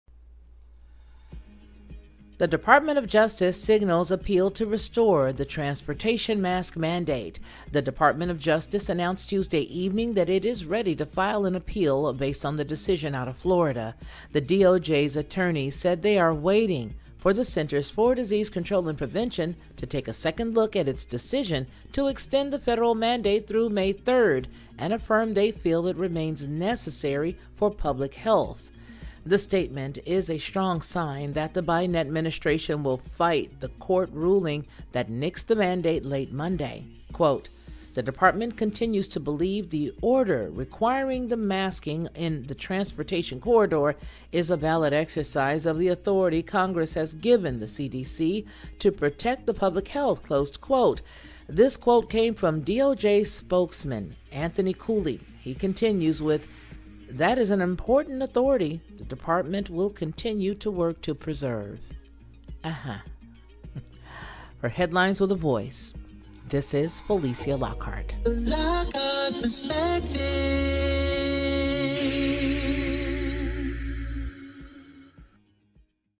DOJ says it will only appeal mask mandate ruling if CDC gives go-ahead
DOJ to appeal... only if CDC gives go-ahead

Ms. Lockhart's Music, By Ronnie Mann Productions